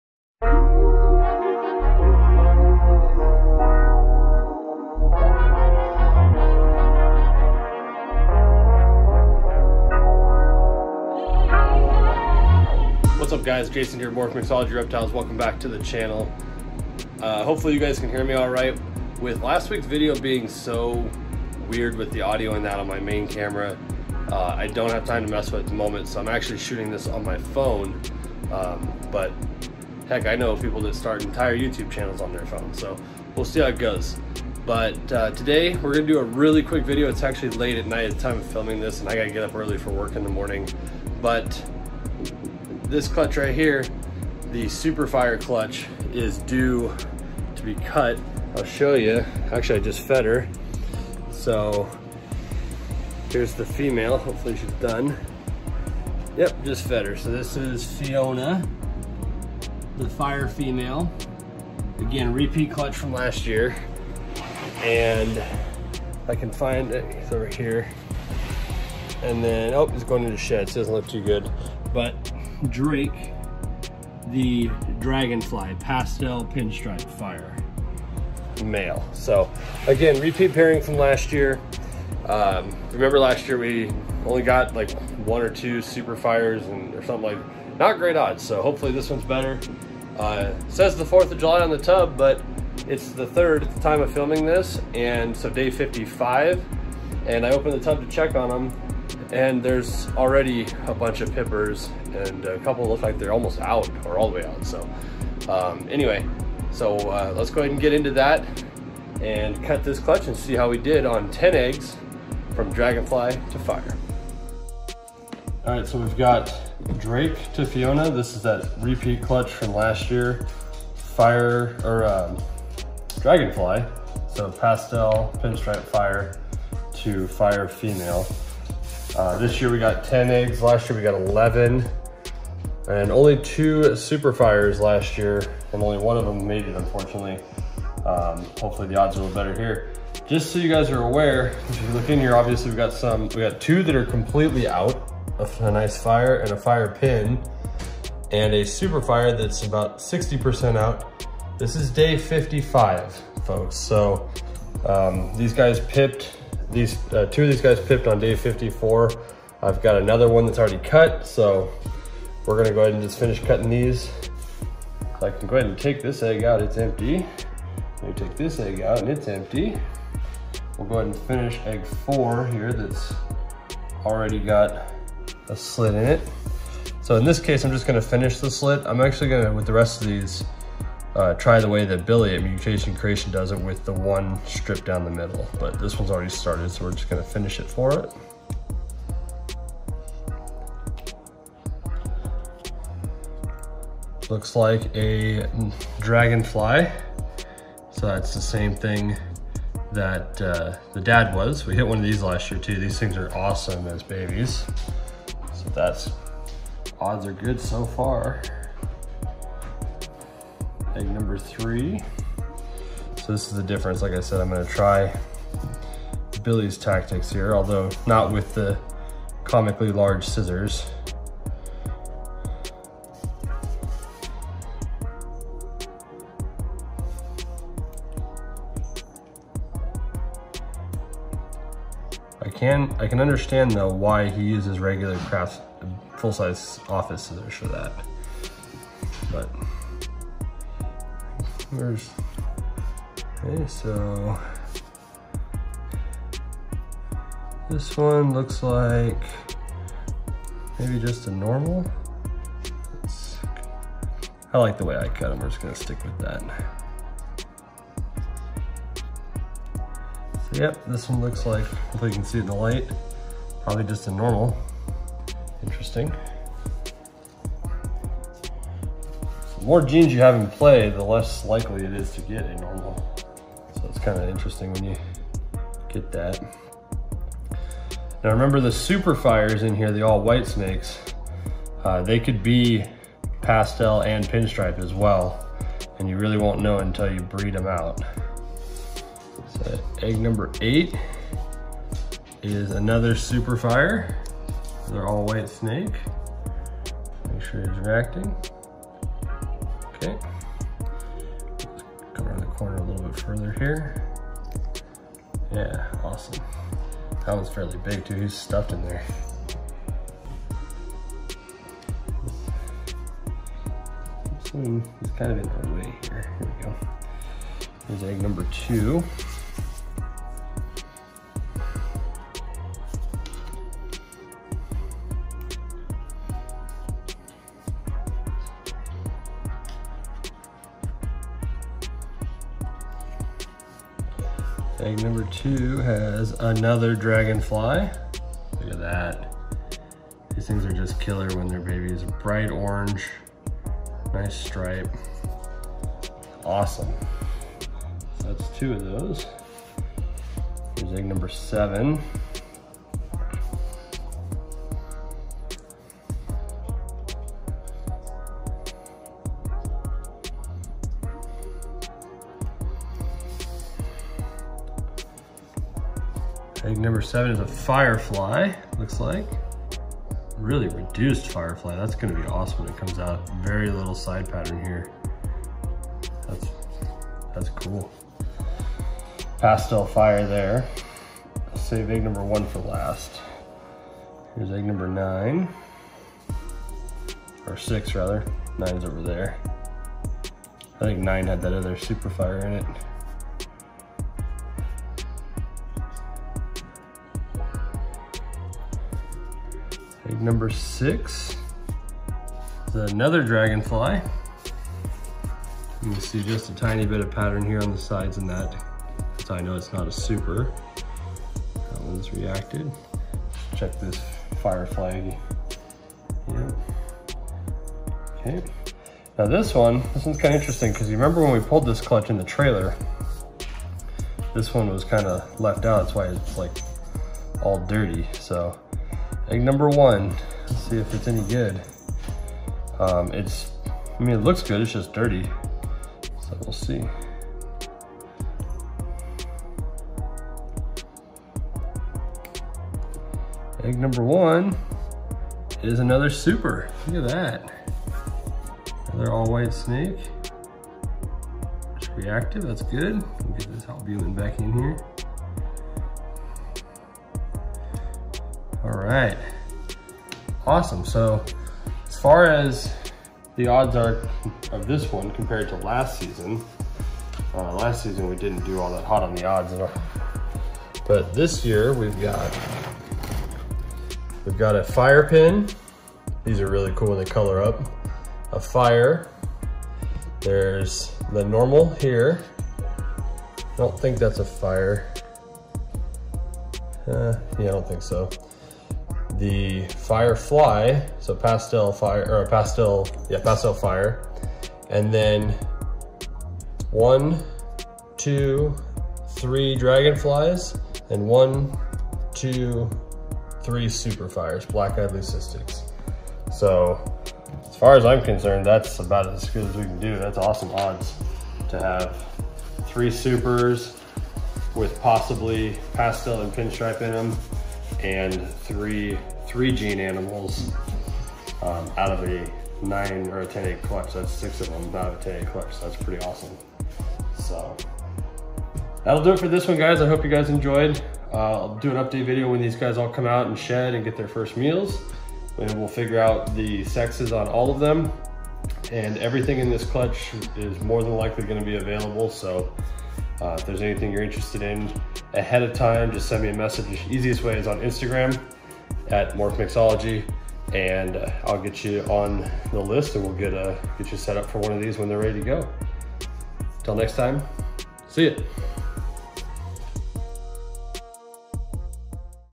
THIS CLUTCH IS STRAIGHT FIRE!!! | CUTTING CLUTCH 4 | Mixology #93
This week we cut Clutch 4. This is a repeat pairing from last year, "Drake" the Dragonfly (Pastel Fire Pinstripe) paired to "Fiona" the Fire. Lets hope the odds are better than last season.

Check Availability here!

Looking to send us something?